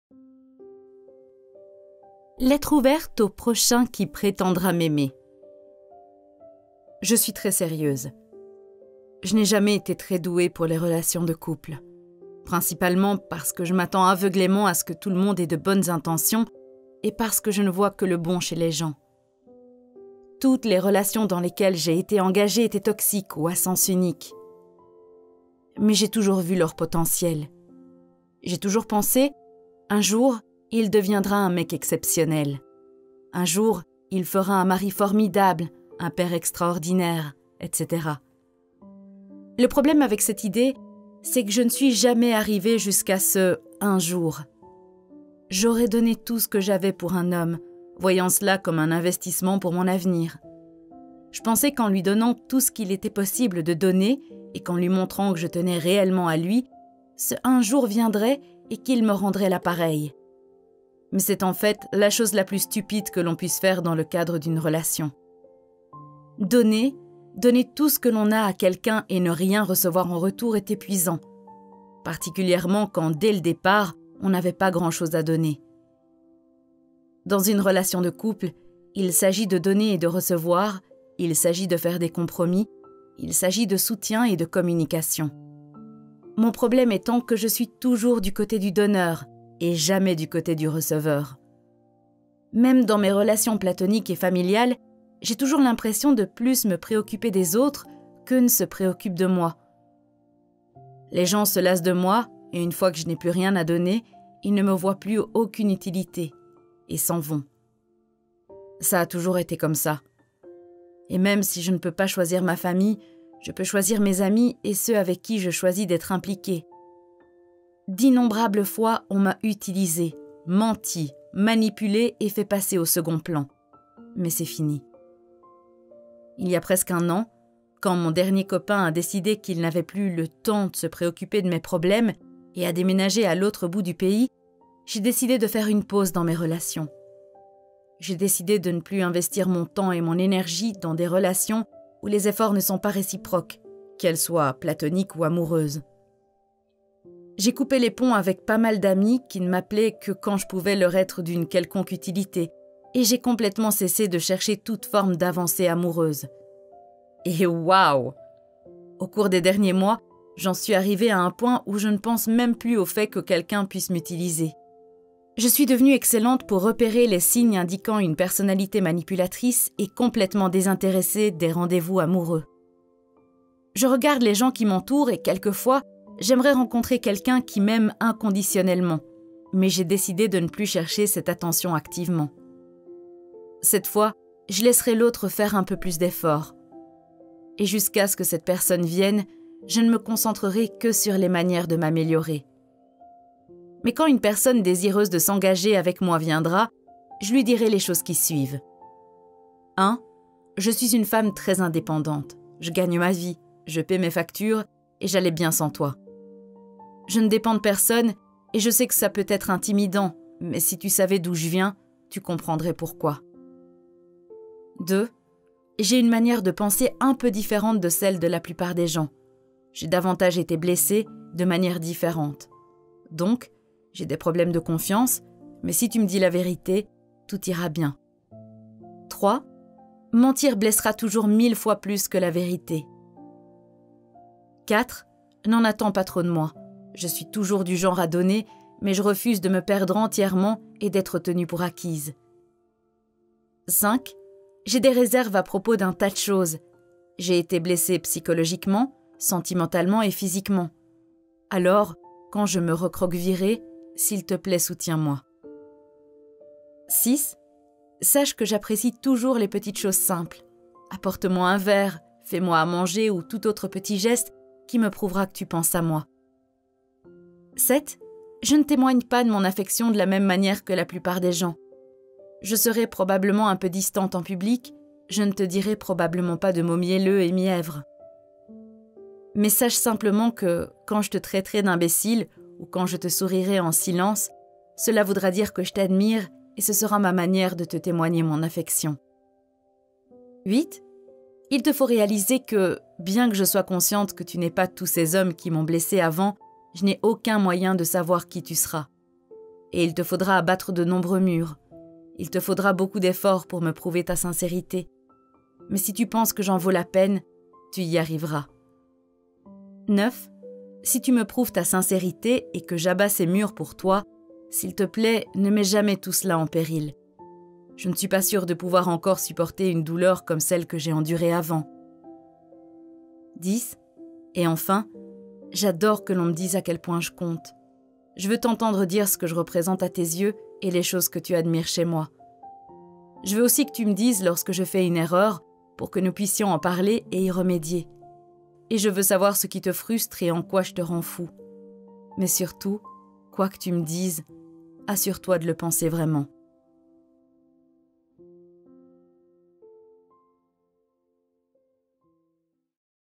Lettre ouverte au prochain qui prétendra m’aimer
Je suis très sérieuse.

Je n’ai jamais été très douée pour les relations de couple. Principalement, parce que je m’attends aveuglément à ce que tout le monde ait de bonnes intentions et parce que je ne vois que le bon chez les gens.

Toutes les relations dans lesquelles j’ai été engagée étaient toxiques ou à sens unique.

Mais j’ai toujours vu leur potentiel. J’ai toujours pensé, « un jour, il deviendra un mec exceptionnel. »

« Un jour, il fera un mari formidable, un père extraordinaire, etc. »

Le problème avec cette idée, c’est que je ne suis jamais arrivée jusqu’à ce « un jour ».